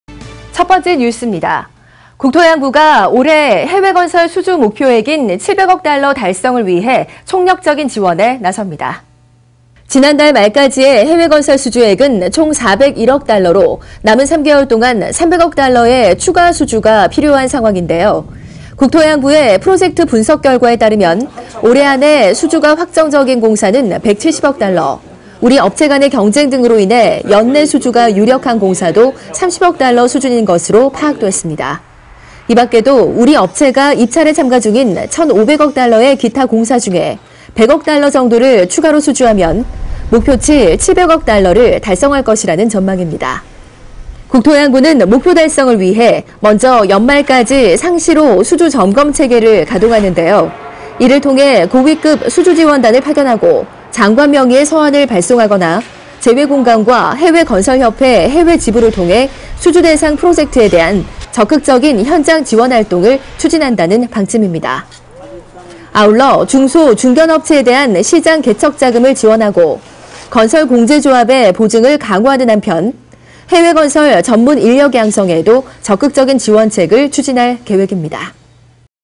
국토부 해외건설 700억 불 수주 총력지원
■국토해양부가 올해 해외건설 수주 목표액인 700억 달러 달성을 위해 총력적인 지원에 나섭니다.

■중소, 중견업체에 대한 시장개척자금을 지원하고 건설공제조합의 보증을 강화하는 한편 해외건설 전문 인력 양성에도 적극적인 지원책을 추진할 계획입니다.